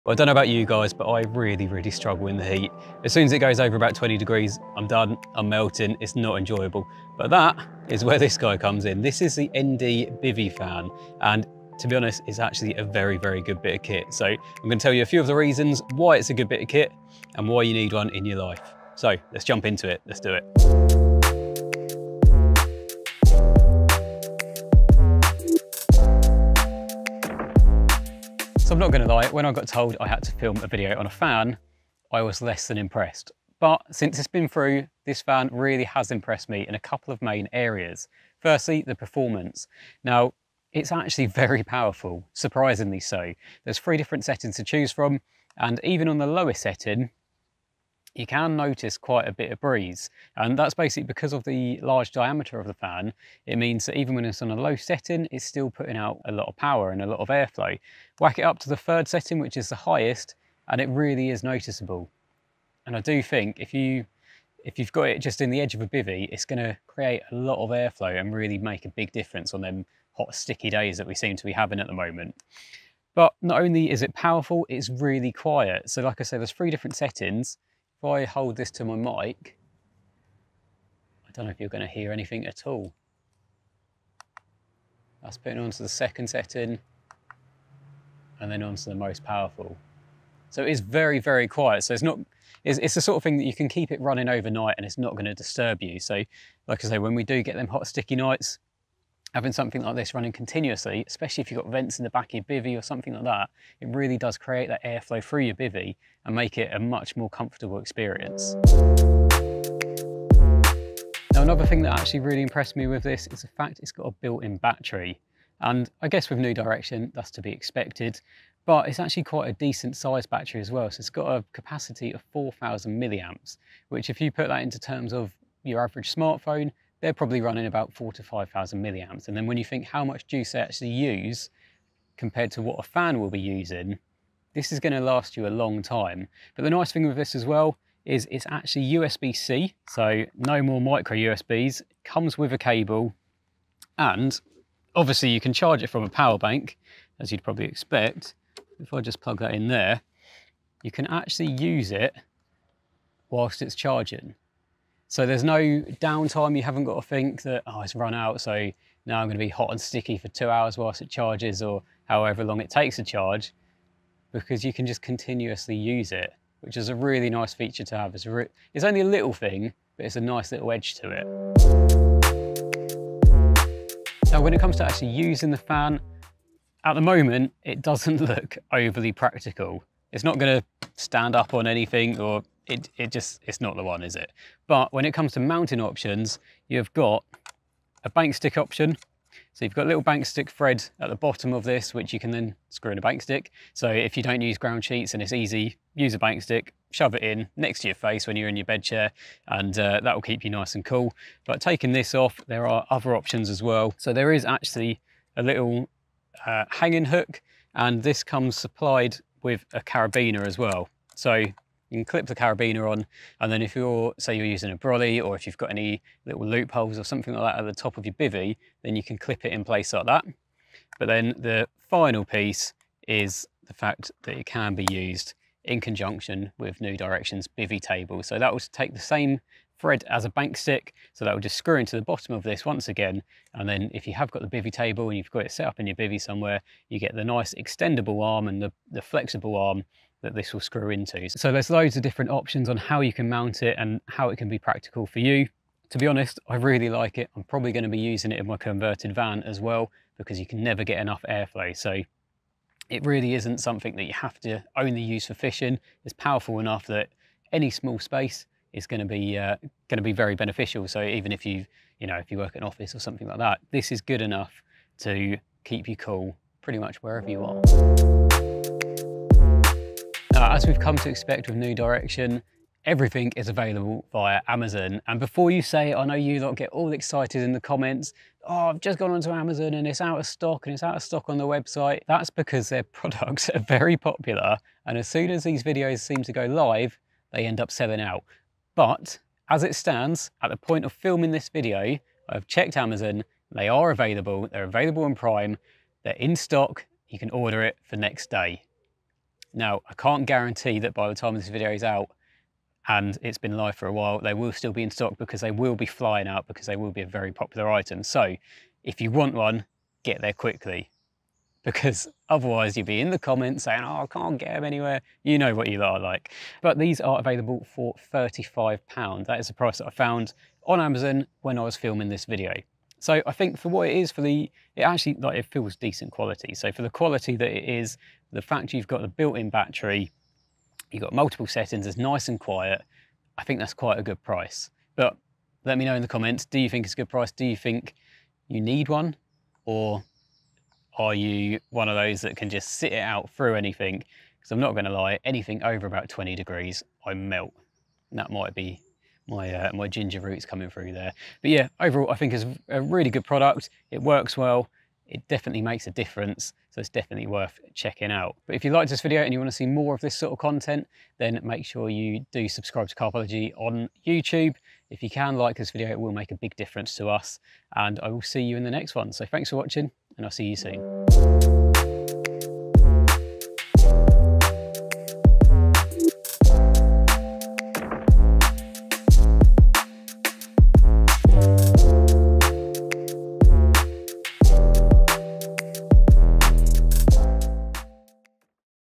BEAT THE HEAT! | New Direction Bivvy Fan
If the harsh summer heat has you struggling, New Direction’s new rechargeable bivvy fan can help. This bivvy fan can help whether you’re travelling across hot regions on a monster Euro adventure, bivvying up on B1 at Linear or even in your own bedroom. It’s portable, easy to travel with, and can be charged and run whilst bankside thanks to your power bank. CARPology’s YouTube presenter and reviewer, Luke Venus, trialled one for a weekend and here’s what he learnt…

𝗜𝗻 𝘁𝗵𝗶𝘀 𝘃𝗶𝗱𝗲𝗼:
0:00 New Direction Bivvy Fan
0:29 Performance
1:13 Quiet
1:48 How is it powered?
2:53 How do I use it?
4:28 How do I get one?

𝗪𝗜𝗡 𝗕𝗜𝗚! £1,000s worth of carp fishing prizes to be won each week, and tickets start from as little as 99p!